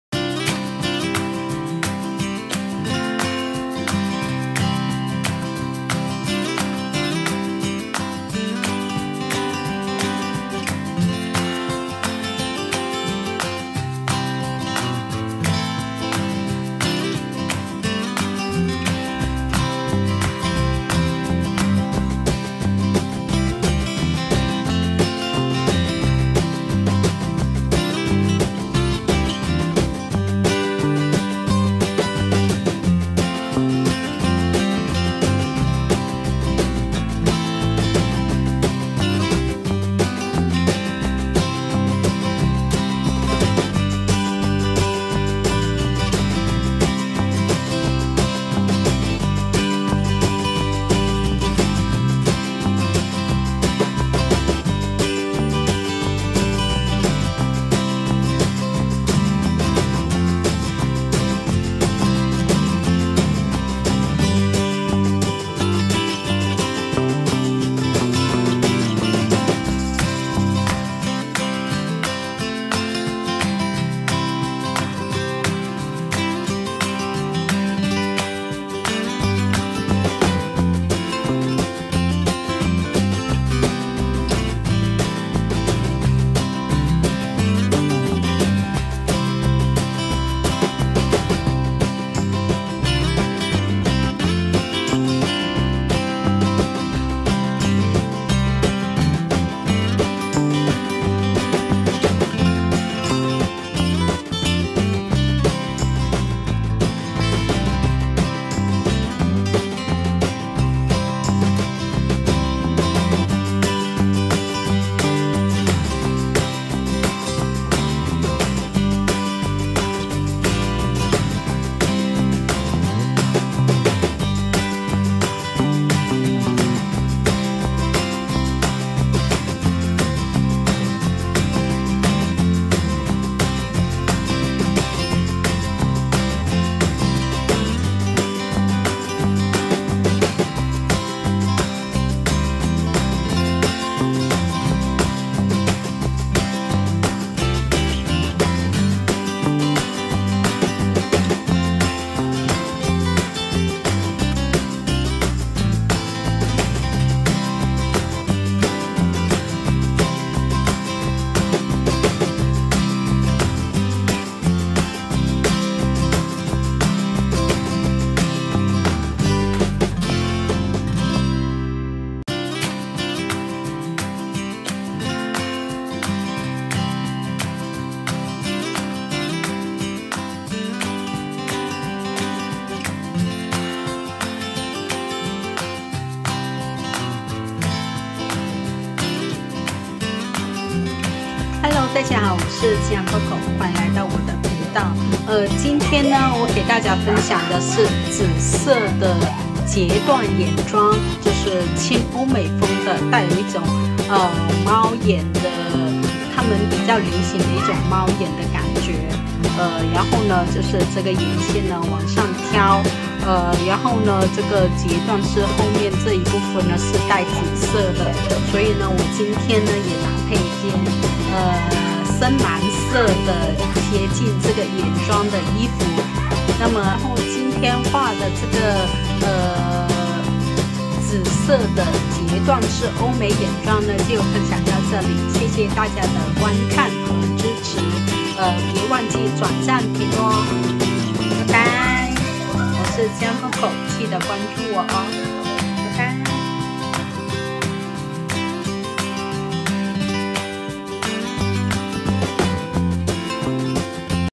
紫色梦幻轻欧式妆容|截断式眼妆
美妆#欧美妆# 紫色梦幻轻欧式妆容分享，非常有气场的一款妆容，特别有些女孩纸有蓝色，紫色情结的可以试下😉 在家无事可以这样化！给人一种梦幻的紫色蝴蝶兰感觉！非常好看！😄咨燃coco美妆频道#